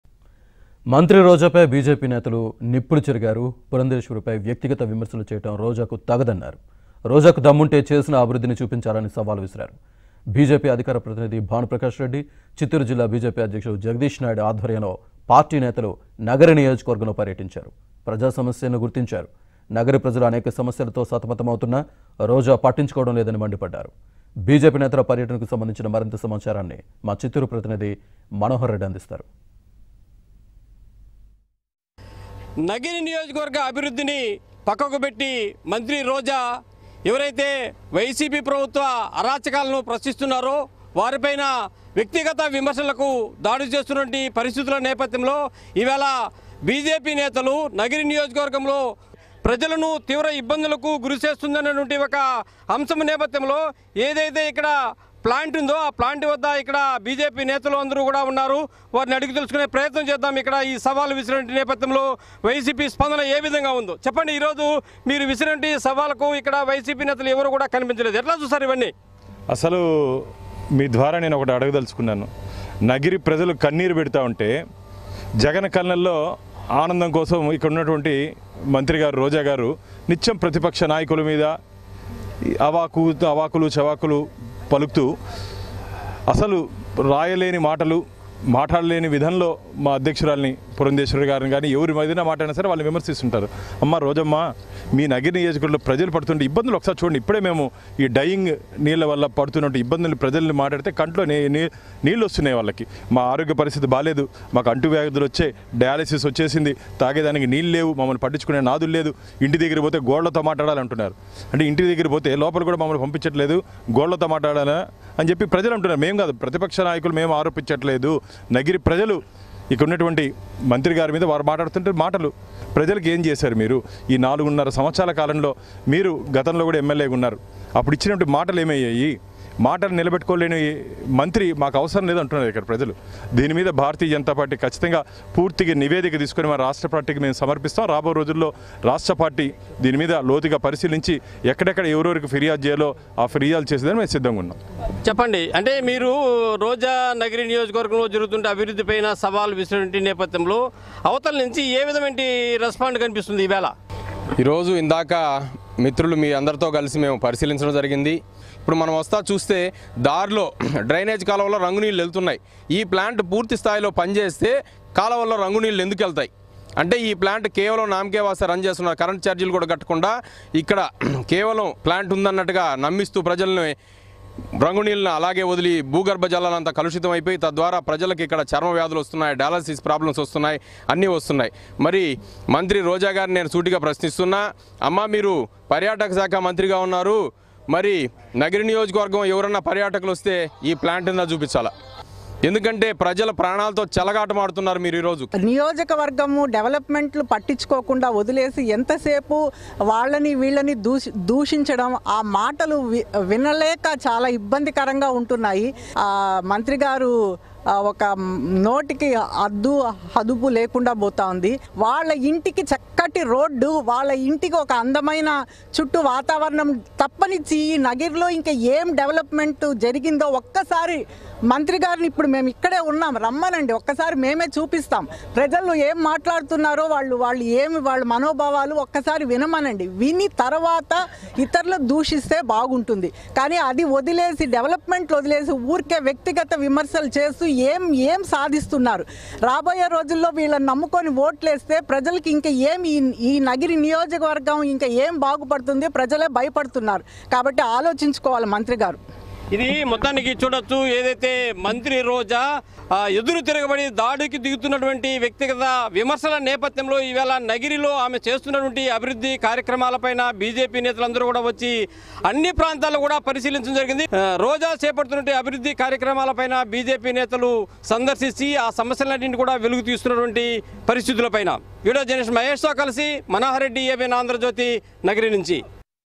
నోరు అదుపులో పెట్టుకో.. మంత్రి రోజా పై నిప్పులు చెరిగిన బీజేపీ నేతలు | AP BJP Leaders Fires On Roja
నోరు అదుపులో పెట్టుకో.. మంత్రి రోజా పై నిప్పులు చెరిగిన బీజేపీ నేతలు | AP BJP Leaders Fires On Roja | ABN Telugu